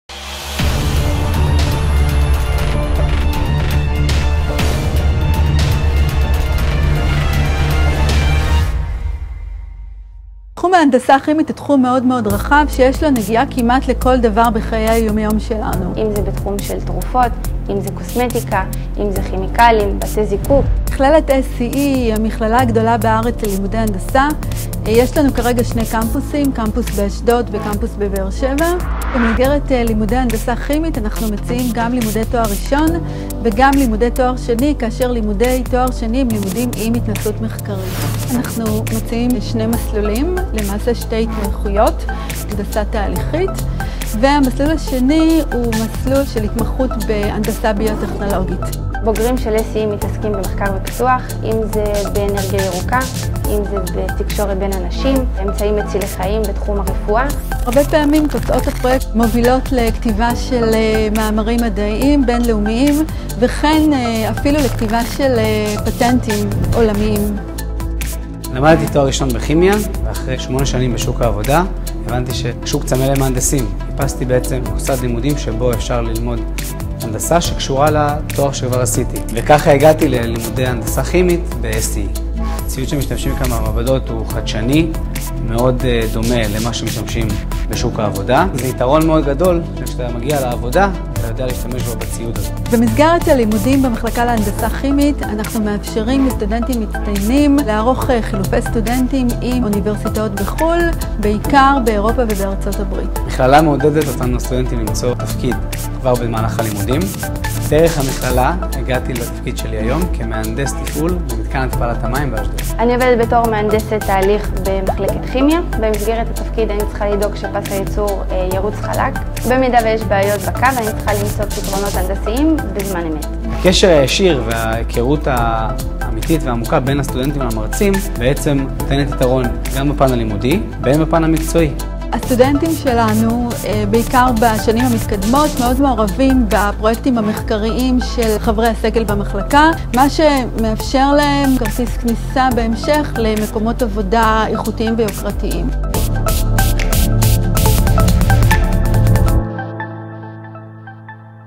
המחלקה להנדסה כימית
המחלקה להנדסה כימית  של SCE מציעה בשני הקמפוסים, באשדוד ובבאר שבע, לימודי תואר ראשון בהנדסה כימית (.B.Sc) ותואר שני עם תזה בהנדסה ירוקה (.M.Sc).